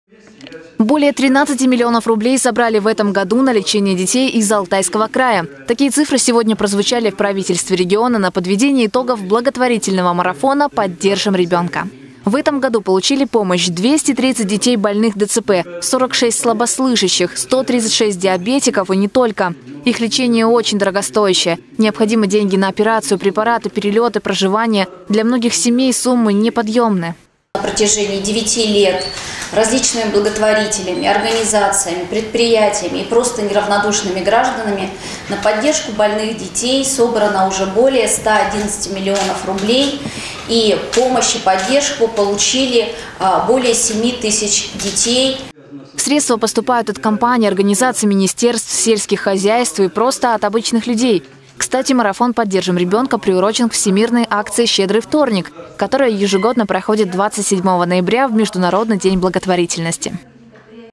На лечение детей из Алтайского края собрали свыше тринадцати миллионов рублей
Такие цифры сегодня прозвучали в правительстве региона на подведении итогов благотворительного марафона «Поддержим ребёнка».